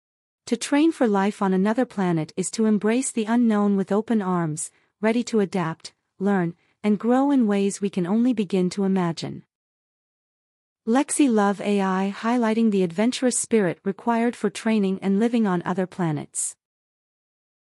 Masterpieces Of Misinterpretation: "Train For Life" - Lexi Love™ AI Fails
Lexi Love™ 🧠🤖😻 To train for life on another planet is to embrace the unknown with open arms, ready to adapt, learn, and grow in ways we can only begin to imagine. Lexi Love AI highlighting the adventurous spirit required for training and living on other planets.